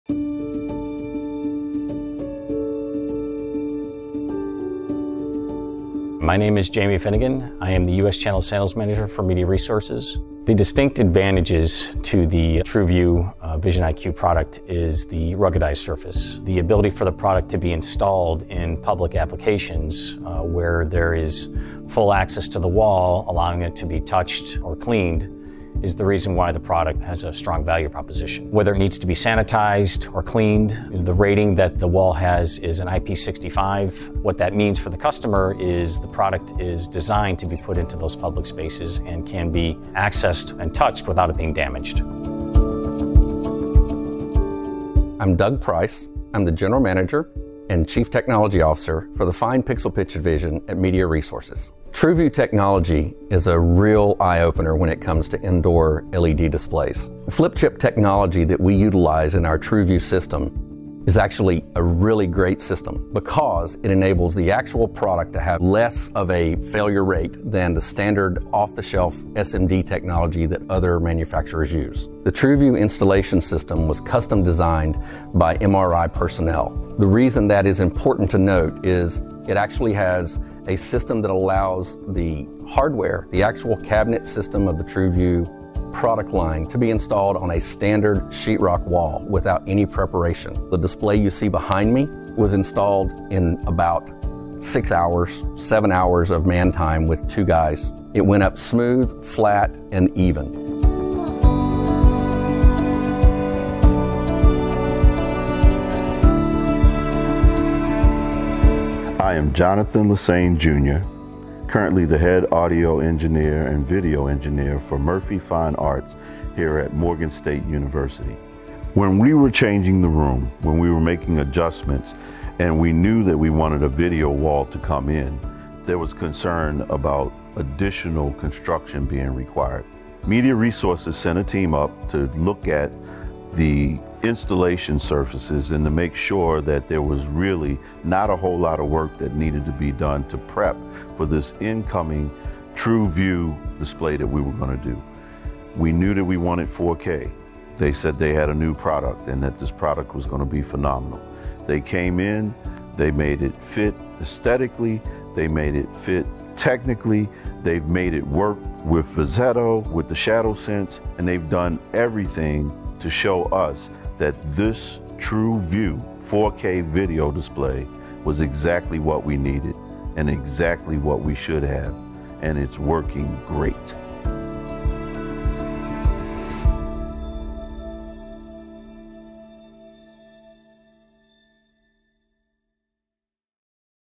TruView Flip Chip COB - MicroLED Displays
Astonish your audience with VISIONiQ TruView - Fine Pixel Pitch Indoor LED

A new standard for indoor displays - with one universal system and a wide range of pixel pitches starting at 0.7mm.

Thank you to Video Supply for helping us put together this video!